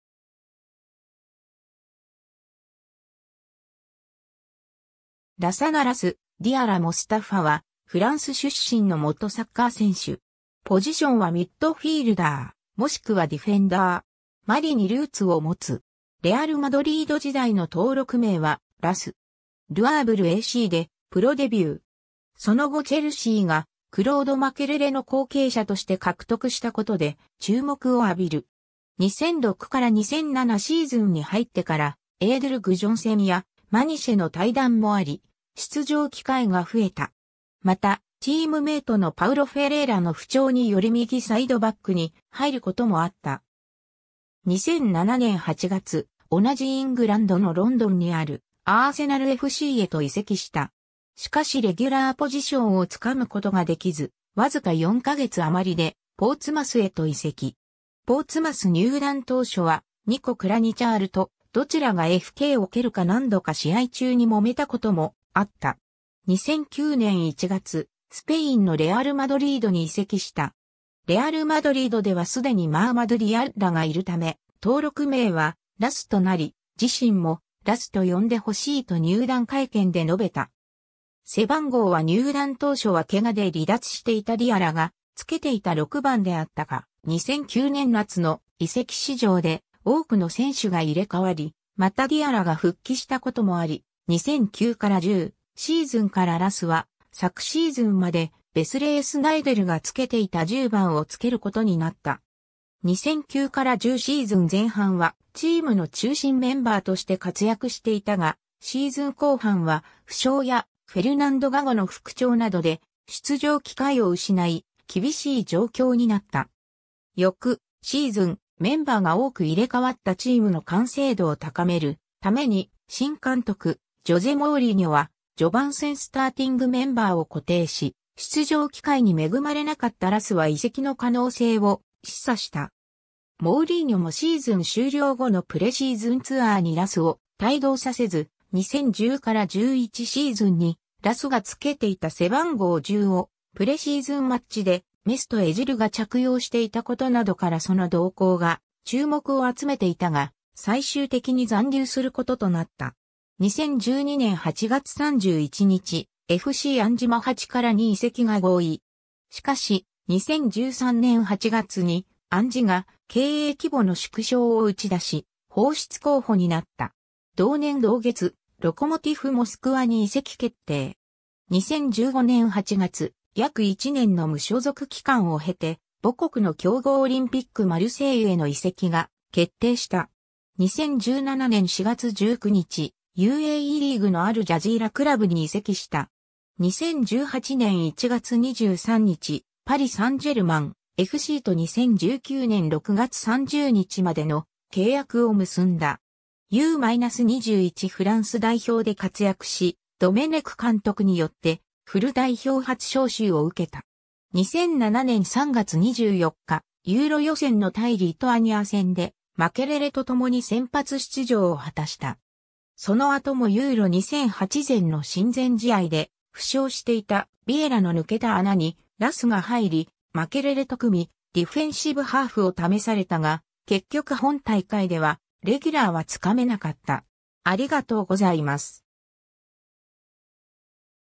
ラッサナ・ディアッラ
ル・アーヴルACの選手
FCアンジ・マハチカラの選手
ラサナ・"ラス"・ディアラ・モスタファ（Lassana "Lass" Diarra Mostafa, 1985年3月10日 - ）は、フランス出身の元サッカー選手。
ポジションはミッドフィールダー、もしくはディフェンダー。
マリにルーツを持つ。
レアル・マドリード時代の登録名は「ラス(Lass)」。
 ル・アーヴルACでプロデビュー。
その後チェルシーがクロード・マケレレの後継者として獲得したことで注目を浴びる。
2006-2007シーズンに入ってからエイドゥル・グジョンセンやマニシェの退団もあり出場機会が増えた。
また、チームメイトのパウロ・フェレイラの不調により右サイドバックに入ることもあった。
 2007年8月、同じイングランドのロンドンにある、アーセナルFCへと移籍した。
しかしレギュラーポジションを掴むことができず、わずか4ヶ月あまりでポーツマスへと移籍。
ポーツマス入団当初はニコ・クラニチャールと、どちらがFKを蹴るか何度か試合中にもめたこともあった。
 2009年1月、スペインのレアル・マドリードに移籍した。
レアル・マドリードでは既にマアマドゥ・ディアッラがいる為、登録名はLASS（ラス）となり、自身もラスと呼んで欲しいと入団会見で述べた。
背番号は入団当初は怪我で離脱していたディアラがつけていた6番であったが、2009年夏の移籍市場で多くの選手が入れ替わり、またディアラが復帰したこともあり、2009-10シーズンからラスは昨シーズンまでヴェスレイ・スナイデルがつけていた10番をつけることになった。
2009-10シーズン前半はチームの中心メンバーとして活躍していたが、シーズン後半は負傷やフェルナンド・ガゴの復調などで出場機会を失い、厳しい状況になった。
 翌シーズン、メンバーが多く入れ替わったチームの完成度を高めるために、新監督ジョゼ・モウリーニョは序盤戦スターティングメンバーを固定し、出場機会に恵まれなかったラスは移籍の可能性を示唆した。
モウリーニョもシーズン終了後のプレシーズンツアーにラスを帯同させず、2010-11シーズンにラスがつけていた背番号10をプレシーズンマッチでメスト・エジルが着用していたことなどからその動向が注目を集めていたが、最終的に残留することとなった。
 2012年8月31日、FCアンジ・マハチカラに移籍が合意。
 しかし、2013年8月にアンジが経営規模の縮小を打ち出し、放出候補になった。
同年同月、ロコモティフ・モスクワに移籍決定。
 2015年8月、約1年の無所属期間を経て、母国の強豪オリンピック・マルセイユへの移籍が決定した。
 2017年4月19日、UAEリーグのアル・ジャジーラ・クラブに移籍した。
 2018年1月23日、パリ・サンジェルマンFCと2019年6月30日までの契約を結んだ。
 U-21フランス代表で活躍し、ドメネク監督によってフル代表初招集を受けた。
2007年3月24日、ユーロ予選の対リトアニア戦でマケレレとともに先発出場を果たした。
その後もEURO2008前の親善試合で、負傷していたヴィエラの抜けた穴にラスが入り、マケレレと組みディフェンシブハーフを試されたが、結局本大会ではレギュラーは掴めなかった。